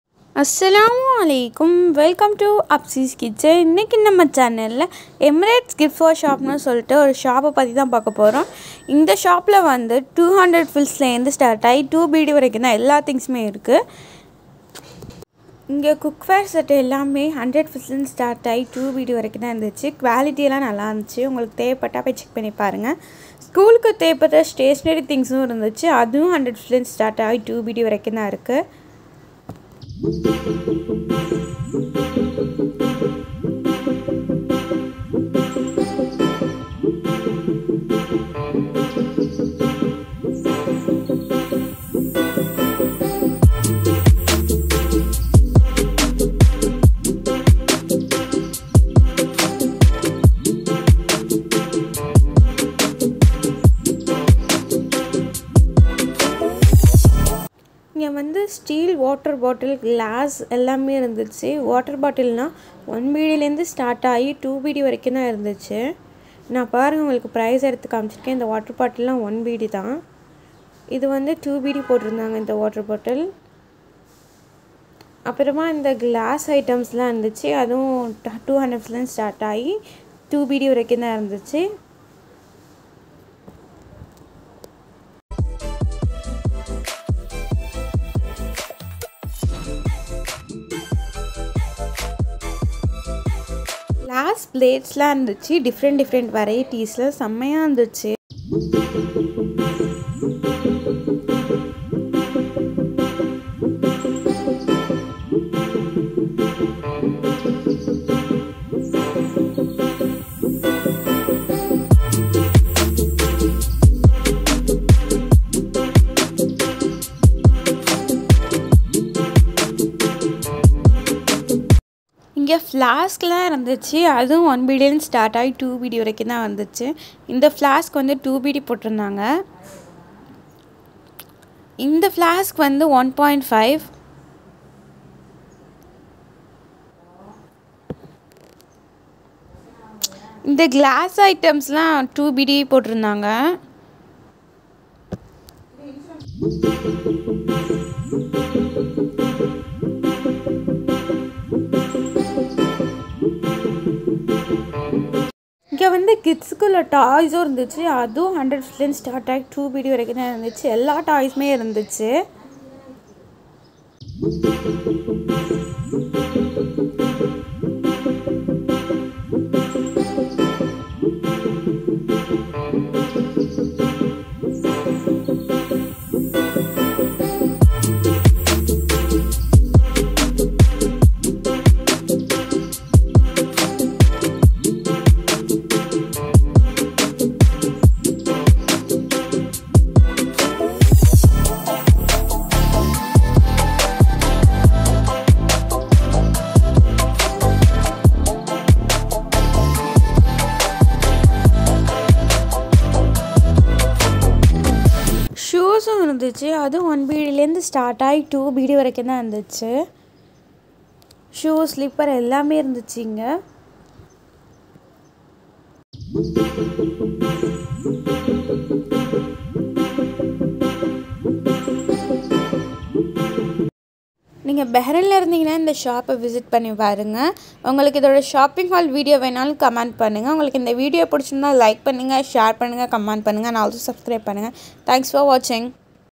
100 fils to 2 dinar shop / Emirates gift for shop bahrain riffa
100 fils to 2 dinar/ Emirates gift for shop bahrain riffa

Best cheapest price shop bahrain
    Emirates gift for shop
price starting from 100 fils to 2 dinar

in Bahrain there is  branches
   riffa bukawara
   sitra highway

huge hand bag, shoes, makeup , watch, dress much more items very cheap price
staring from 100 fils to 2 dinar